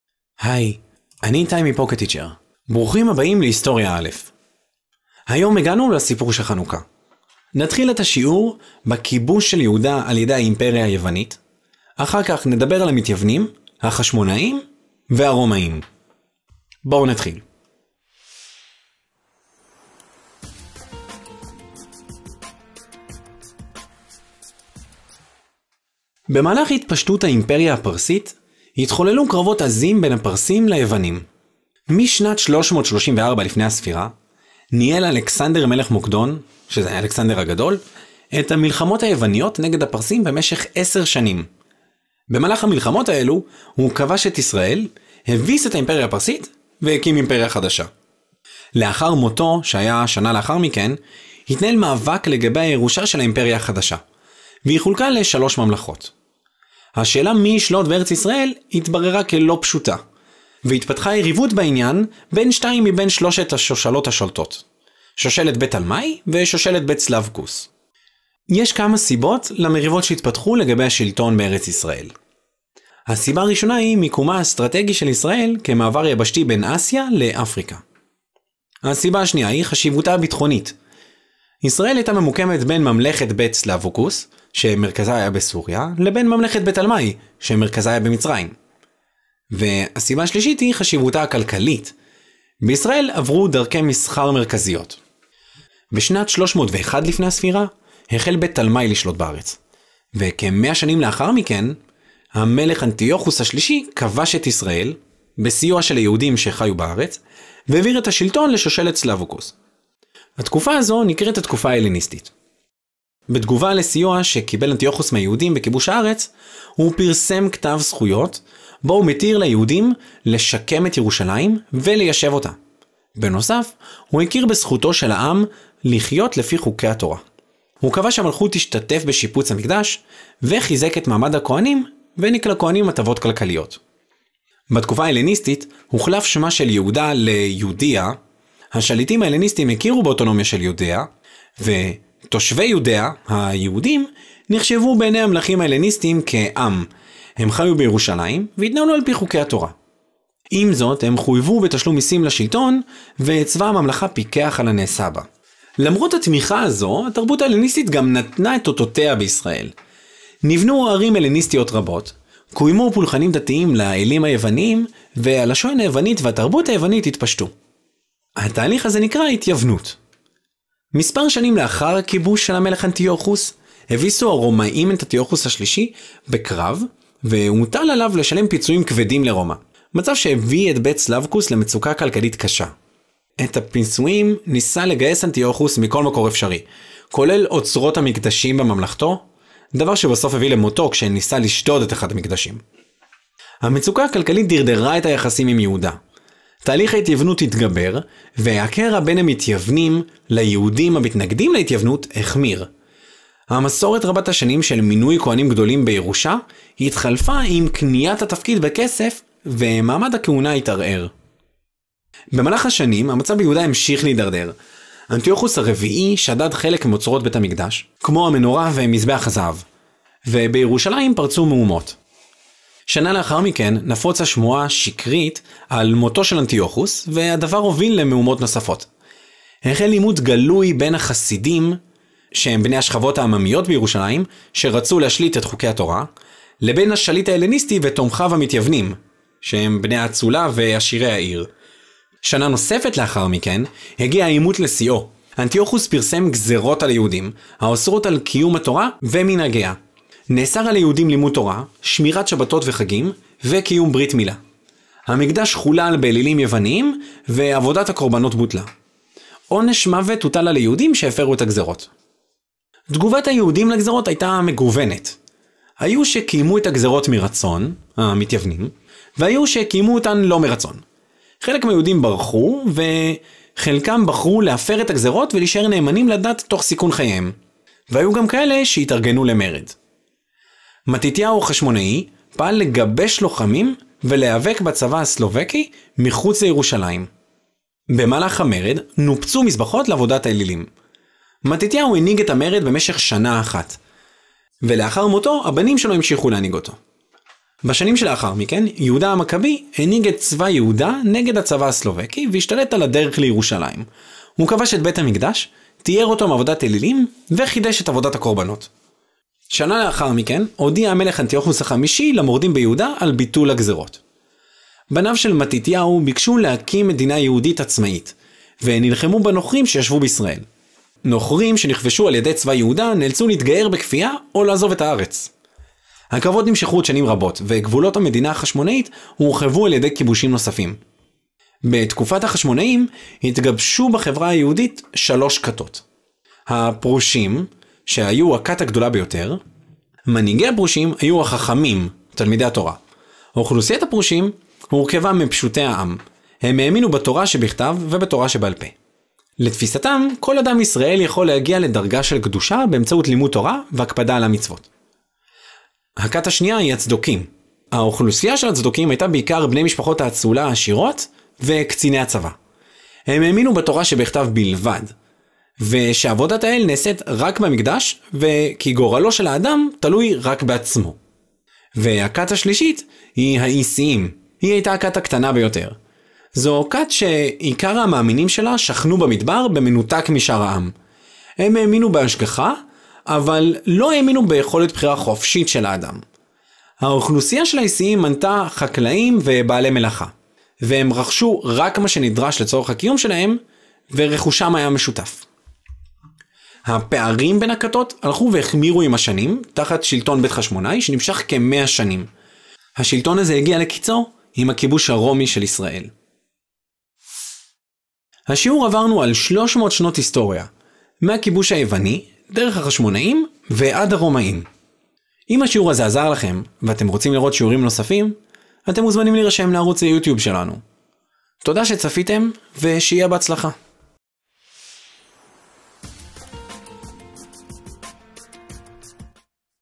היסטוריה לבגרות - ממדינת מקדש לעם הספר – ממדינת מקדש לממלכה ריבונית
החשמונאים, יהודה המכבי ועוד סיפורי חנוכה
מה נלמד בסרטון?

 נלמד על כיבוש יהודה ע"י האימפריה היוונית, נדבר על המתיוונים, החשמונאים והרומאים.

במהלך השיעור נעבור על מספר אירועים מרכזיים:
במהלך התפשטות האימפריה הפרסית התחוללו קרבות עזים בין הפרסים ליוונים. משנת 334 לפנה"ס ניהל אלכסנדר מלך מוקדון (אלכסנדר הגדול) את המלחמות היווניות נגד הפרסים במשך 10 שנים. במהלך מלחמות אלו כבש את ישראל, הביס את האימפריה הפרסית והקים אימפריה חדשה.
לאחר מותו של אלכסנדר הגדול השלטון בישראל עבר לשושלת בית תלמי, כ- 100 שנים לאחר מכן המלך אנטיוכוס השלישי כבש את ישראל והעביר את השלטון לשושלת סלווקוס.
במהלך השנים מצב ביהודה התדרדר, אנטיוכוס ה-4 שדד חלק מאוצרות בית המקדש ובירושלים פרצו מהומות. לאחר מכן, הגיע העימות לשיאו, אנטיוכוס פרסם גזרות על היהודים האוסרות על קיום התורה ומנהגיה. נאסר על היהודים לימוד תורה, שמירת שבתות וחגים וקיום ברית מילה. המקדש חולל באלילים יוניים ועבודת הקורבנות בוטלה. עונש מוות הוטל על היהודים שהפרו את הגזרות.
לאור הגזרות, מתתיהו החשמונאי פעל לגבש לוחמים ולמרוד. מתתיהו הנהיג את המרד במשך שנה ולאחר מותו, הבנים שלו המשיכו להנהיג אותו. בשנים שלאחר מכן (יהודה המקבי הנהיג את צבא יהודה נגד הצבא הסלווקי והשתלט על הדרך לירושלים. הוא כבש את בית המקדש, טיהר אותו מעבודת אלילים וחידש את עבודת הקורבנות. שנה לאחר מכן הודיע המלך אנטיוכוס ה-5 למורדים ביהודה על ביטול הגזרות.
בתקופת החשמונאים התגבשו בחברה היהודית שלוש כתות: הפרושים, הצקודים והאיסיים. הפערים בין הכתות הלכו והחמירו עם השנים תחת שלטון בית חשמונאי שנמשך כ-100 שנים. שלטון זה הגיע לקיצו עם כיבוש הרומאים את ישראל.

אם יש לכם שאלות או בקשות אתם מוזמנים להגיב על הסרטון
בהצלחה!